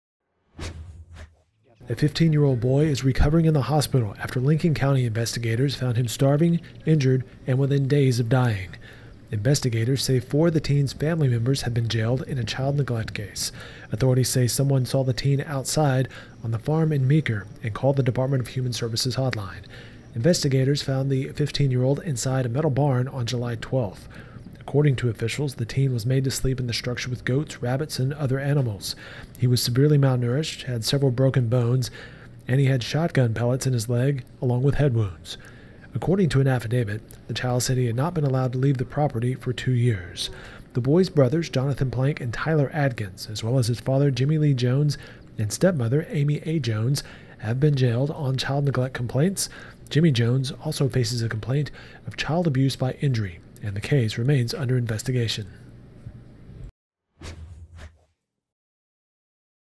Boy found starving
An injured and starving 15-year-old boy was within days of death when investigators found him at a Lincoln County farm